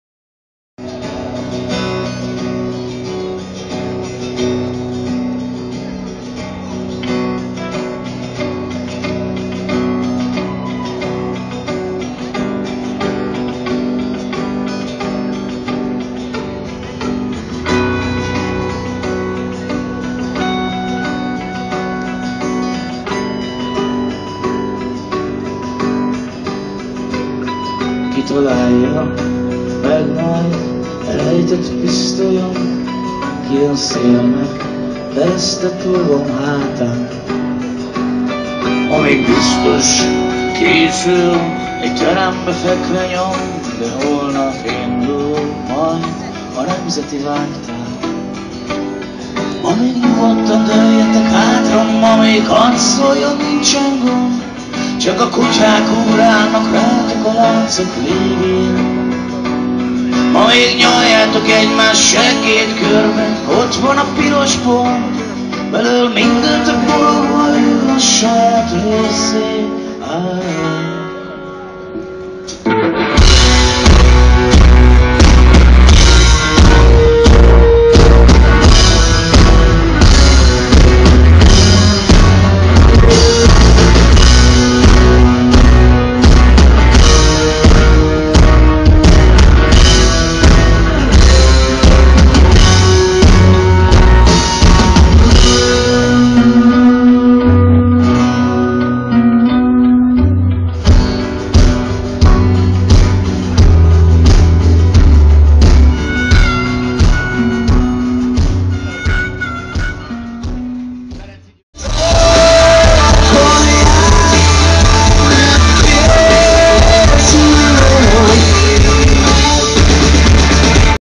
Kiscsillag - Kolozsvári Magyar Napok 2014
Nem kell filmművészeti csodára várni: telefonnal felvett részleteket vágtam össze. Laza hangulatanyag a Kiscsillag Kolozsvári fellépéséről.
Khösziii